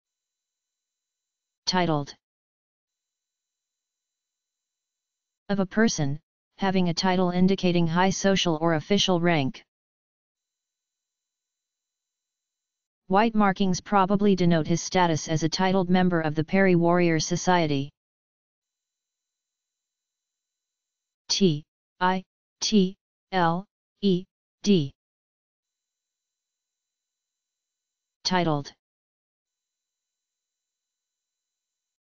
Titled - Meaning and How To Pronounce
Title: titled

(of a person) having a title indicating high social or official rank.

White markings probably denote his status as a titled member of the Peri warrior society.

How to pronounce titled
definition of titled
audio dictionary
How to say titled
What is the meaning of titled
Pronounce titled